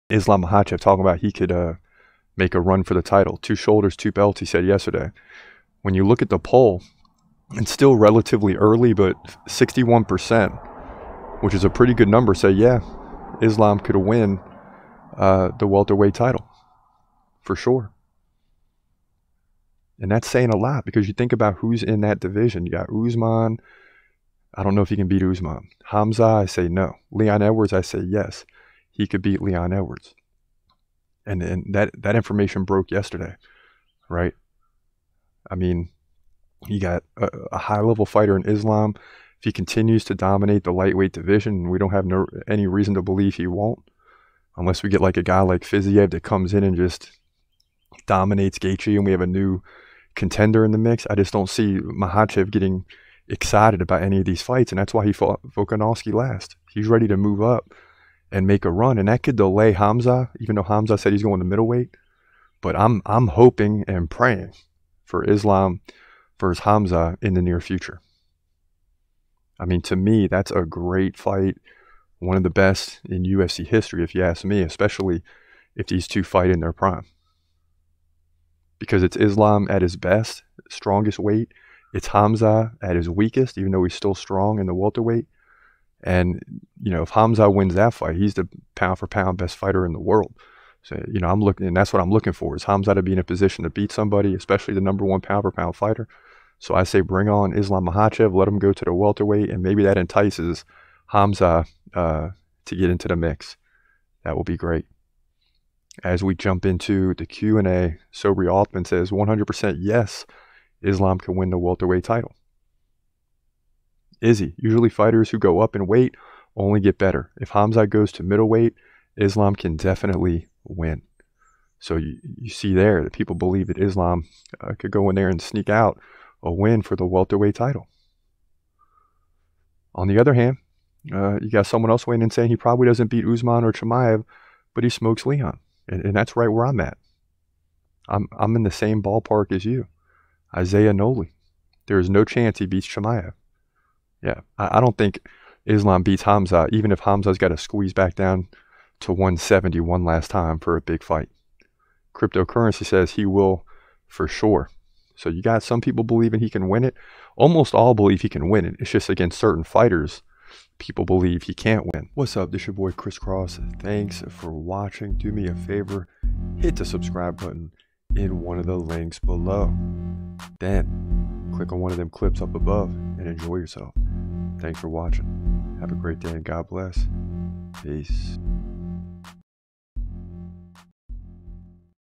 Can Islam Makhachev Win The Welterweight Title..?
@ChrisCrossMMA reacts to a UFC Poll on whether or not Islam Makhachev can win the welterweight title...